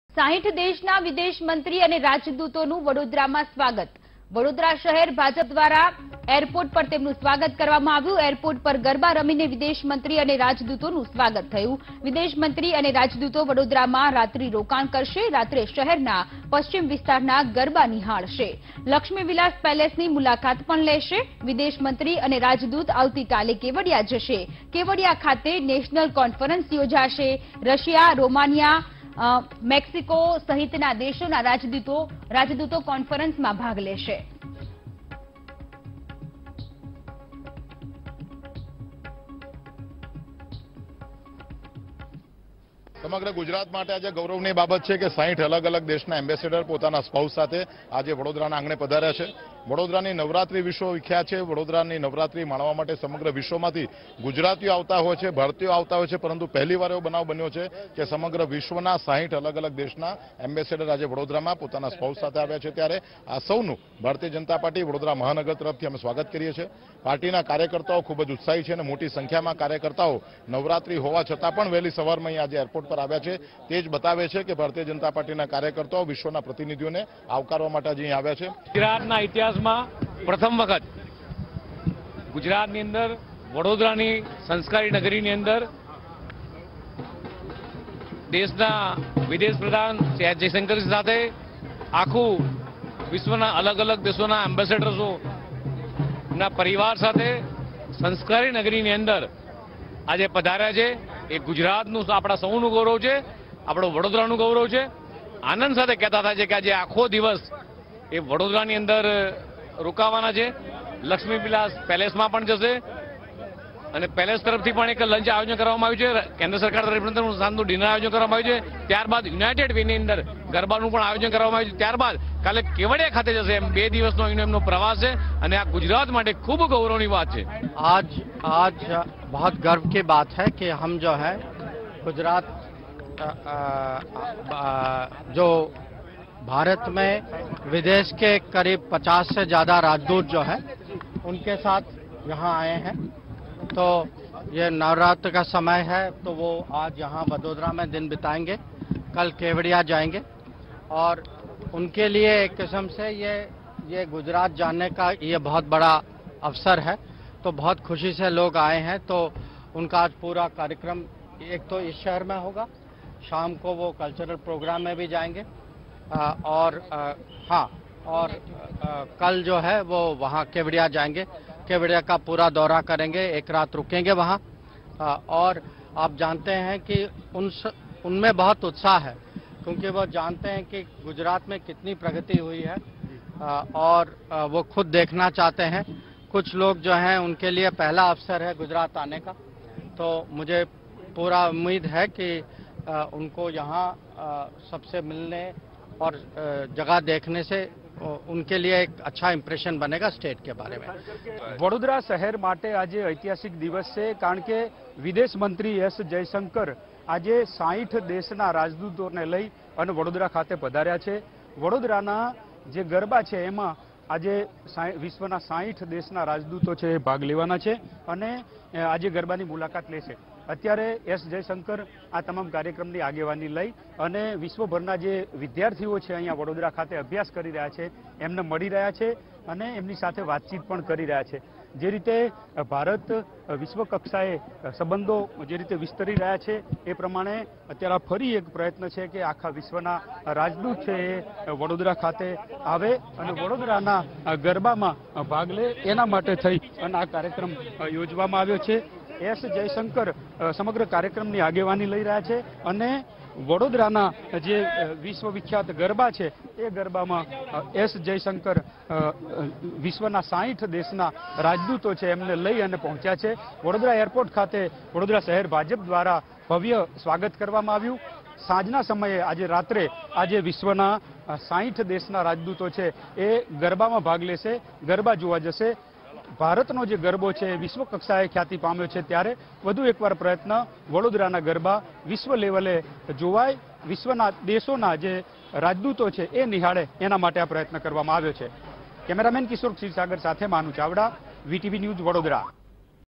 Vadodara : 60 દેશના વિદેશમંત્રી અને રાજદૂતોનું વડોદરામાં સ્વાગત, Kevadia ખાતે યોજાશે નેશનલ કોન્ફરન્સ
Vadodara : 60 દેશના વિદેશમંત્રી અને રાજદૂતોનું વડોદરામાં સ્વાગત, Kevadia ખાતે યોજાશે નેશનલ કોન્ફરન્સ | VTV Gujarati